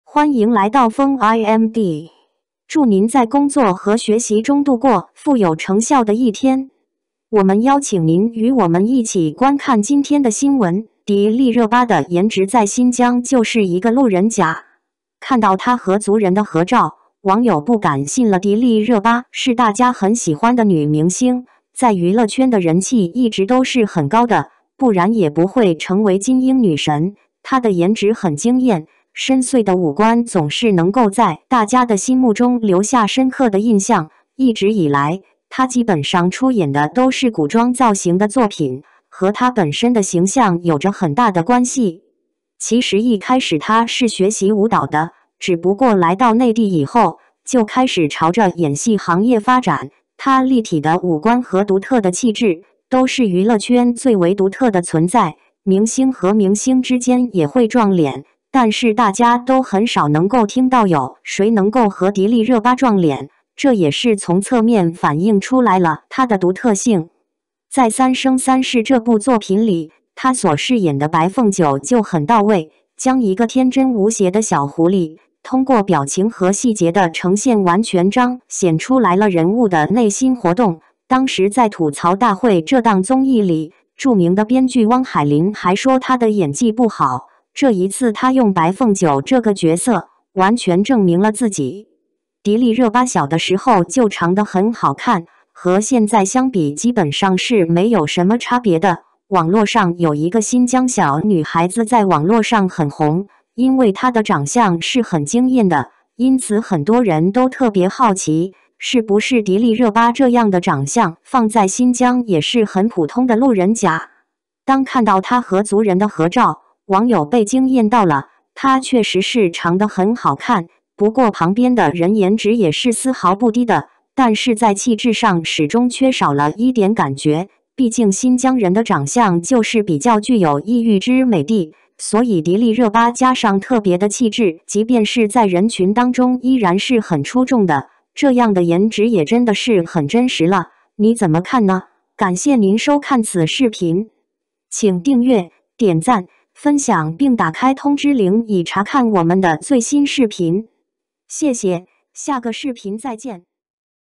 迪丽热巴的颜值在新疆就是一个路人甲？看到她和族人的合照，网友不敢信了
~~~ ENG SUB